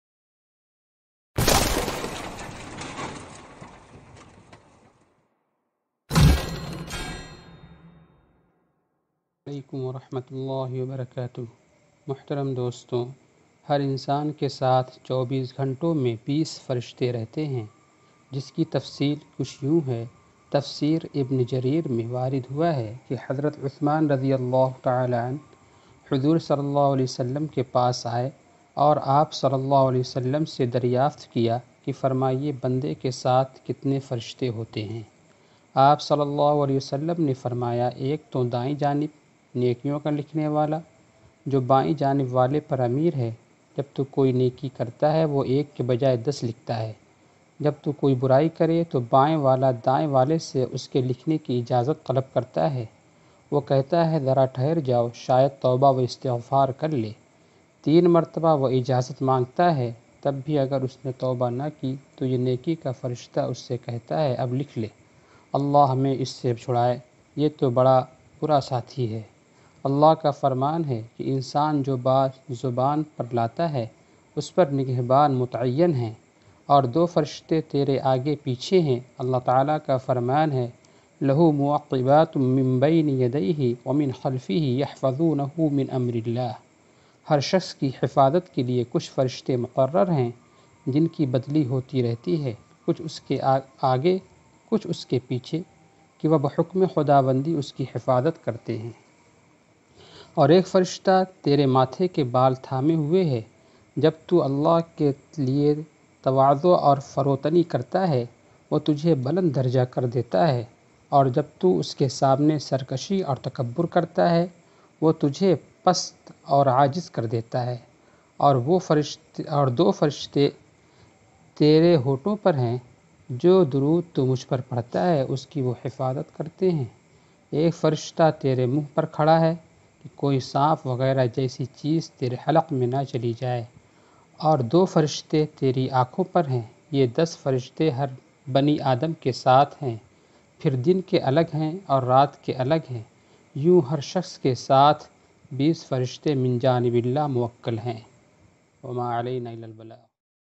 ہر انسان کے ساتھ چوبیس گھنٹوں میں بیس فرشتے رہتے ہیں
میں نے اس ویڈیو میں یہ بتایا ہے کہ ہر انسان
کے ساتھ چوبیس گھنٹوں میں بیس
فرشتے رہتے ہیں